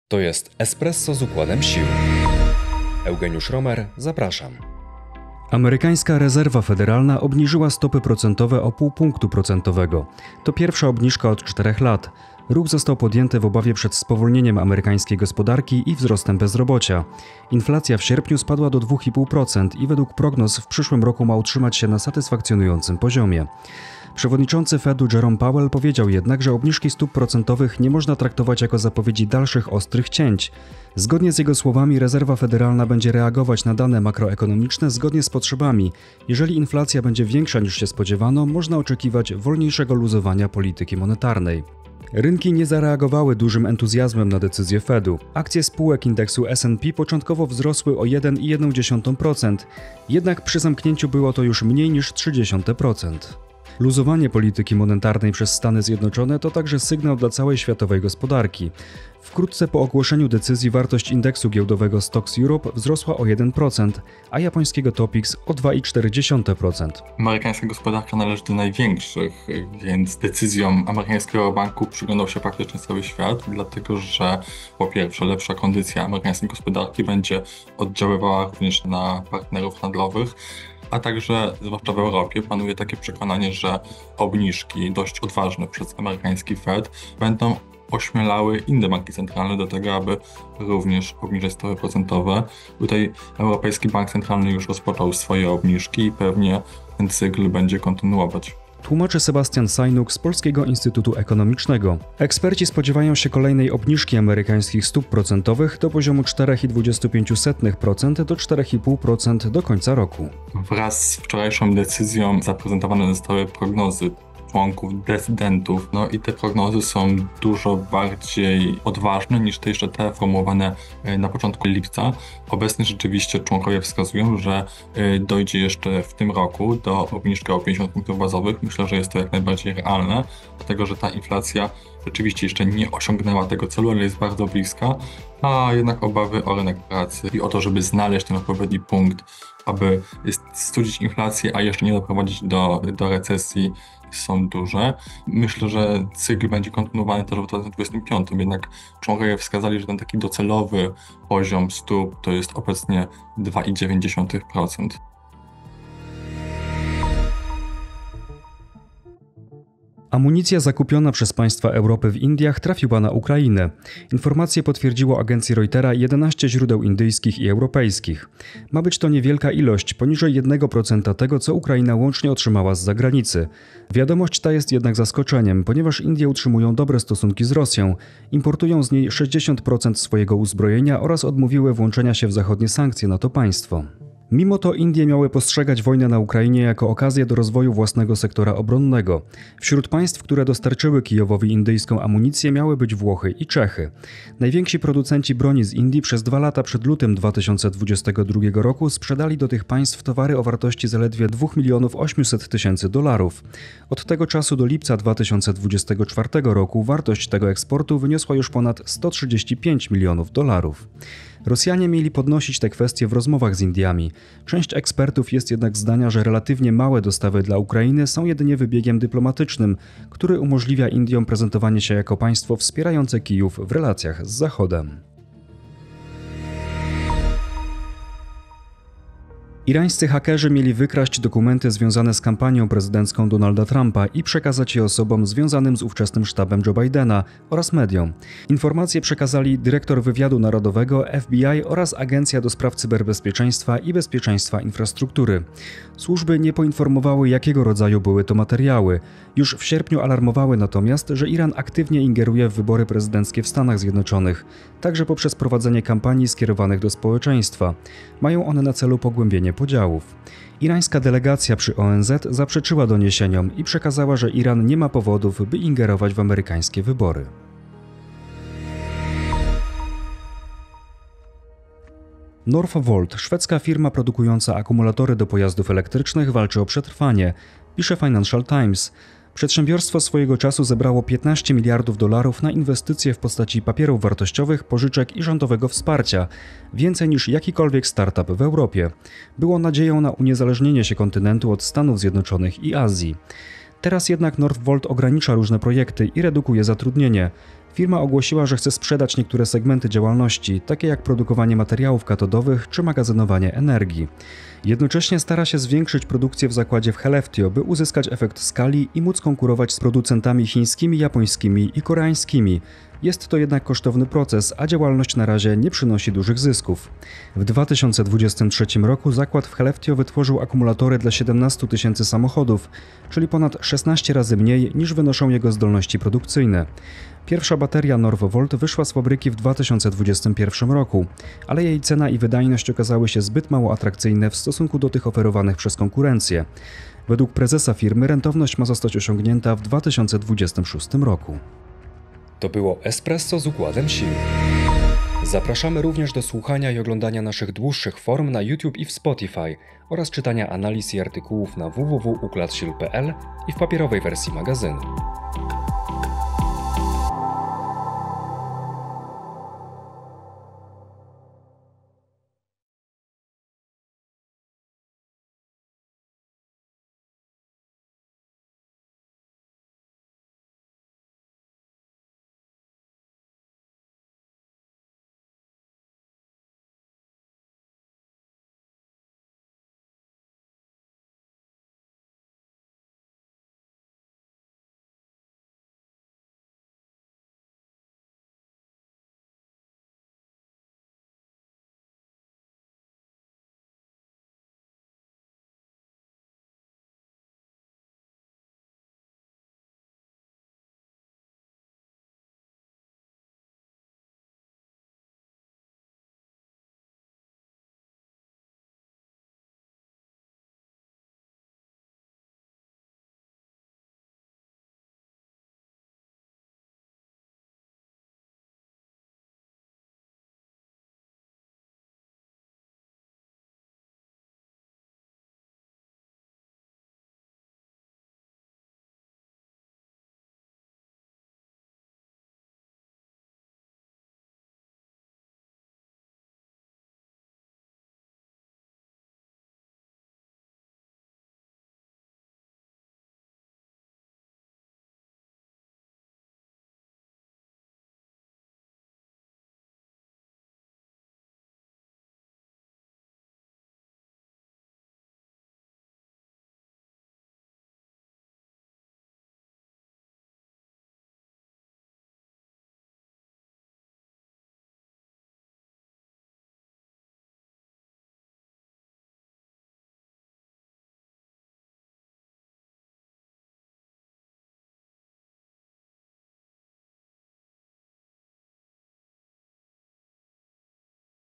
FED obniża stopy procentowe - Eugeniusz Romer, Sebastian Sajnóg
Amerykańska Rezerwa Federalna obniżyła stopy procentowe o pół punktu procentowego. To pierwsza obniżka od czterech lat. Ruch został podjęty w obawie przed spowolnieniem amerykańskiej gospodarki i wzrostem bezrobocia. Inflacja w sierpniu spadła do 2,5 proc. i według prognoz w przyszłym roku ma utrzymać się na satysfakcjonującym poziomie.

Przewodniczący FEDu Jerome Powell powiedział jednak, że obniżki stóp procentowych nie można traktować jako zapowiedzi dalszych ostrych cięć. Zgodnie z jego słowami, Rezerwa Federalna będzie reagować na dane makroekonomiczne zgodnie z potrzebami – jeżeli inflacja będzie większa niż się spodziewano, można oczekiwać wolniejszego luzowania polityki monetarnej.

Rynki nie zareagowały dużym entuzjazmem na decyzję FEDu. Akcje spółek indeksu S&P 500 początkowo wzrosły o 1,1 proc., jednak przy zamknięciu było to już mniej niż 0,3 proc.

Luzowanie polityki monetarnej przez Stany Zjednoczone to także sygnał dla całej światowej gospodarki. Wkrótce po ogłoszeniu decyzji wartość indeksu giełdowego Stoxx Europe 600 wzrosła o 1 proc., a japońskiego Topix o 2,4 proc.

Decyzji FEDu przyglądał się cały świat. Lepsza kondycja gospodarki USA będzie wpływała na partnerów handlowych, a także będzie sygnałem dla innych banków centralnych, by obniżały stopy procentowe. Europejski Bank Centralny już rozpoczął własne obniżki

- tłumaczy Sebastian Sajnóg z Polskiego Instytutu Ekonomicznego.